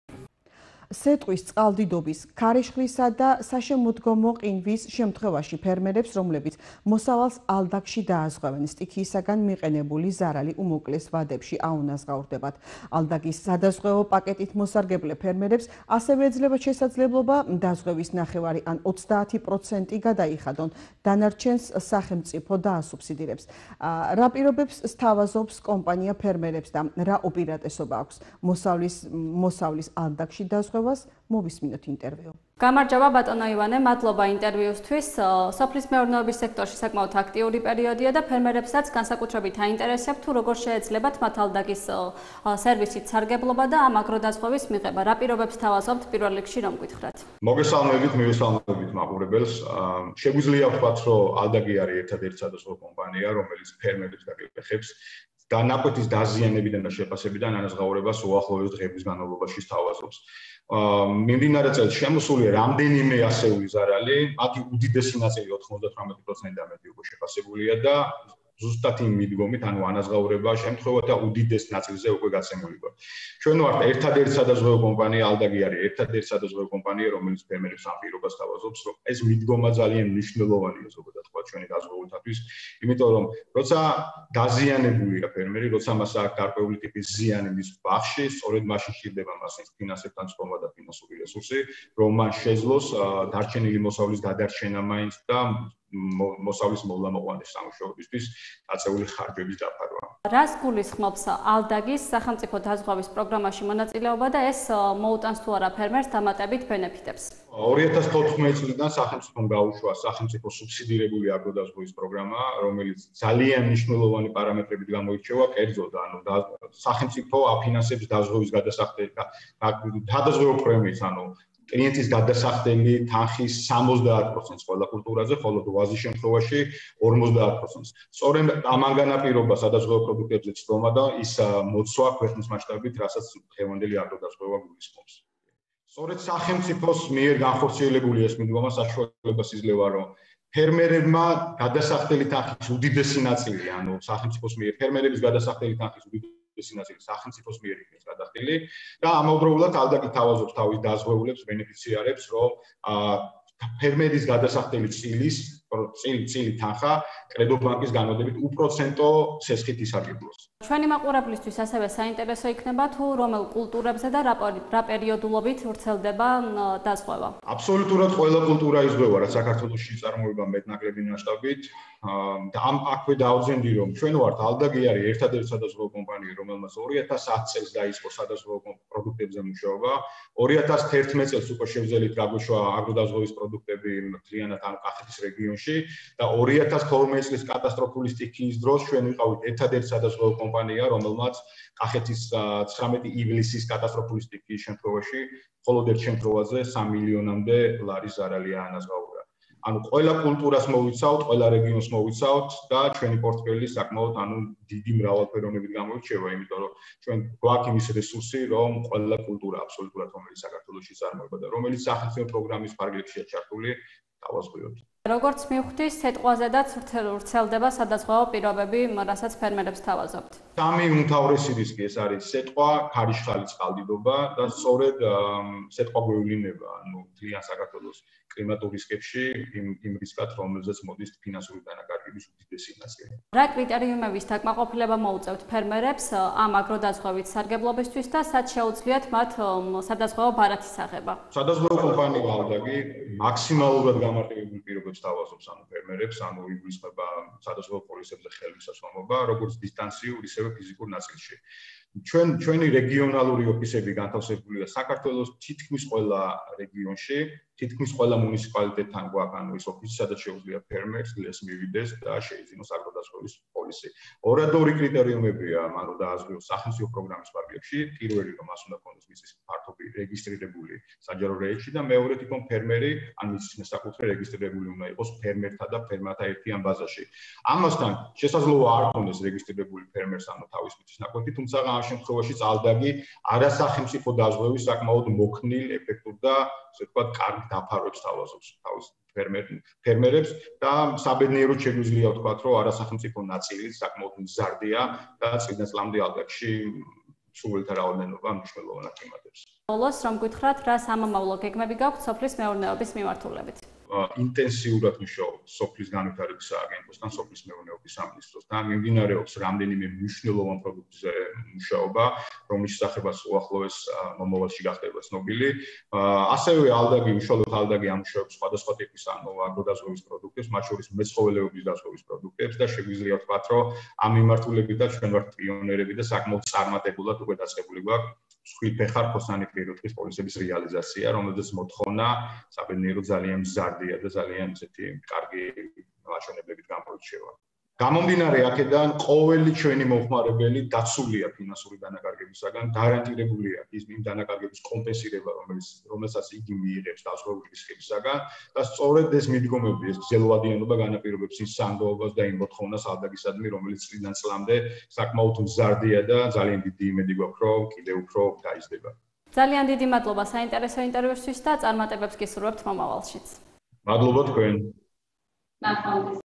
რა უპირატესობა აქვს მოსავლის „ალდაგში“ დაზღვევას? - ინტერვიუ
სეტყვის, წყალდიდობის, ქარიშხლისა და საშემოდგომო ყინვის შემთხვევაში ფერმერებს, რომლებიც  მოსავალს ალდაგში დააზღვევენ, სტიქიისგან მიყენებული ზარალი უმოკლეს ვადებში აუნაზღაურდებათ. ალდაგის სადაზღვევო პაკეტით მოსარგებლე ფერმერებს ასევე ეძლევათ შესაძლებლობა დაზღვევის ნახევარი ან 30 % გადაიხადონ, დანარჩენს სახელმწიფო დაასუბსიდირებს. რას პირობებს სთავაზობს კომპანია ფერმერებს და რა უპირატესობა აქვს მოსავლის ალდაგში დაზღვევას, მოვისმინოთ ინტერვიუ.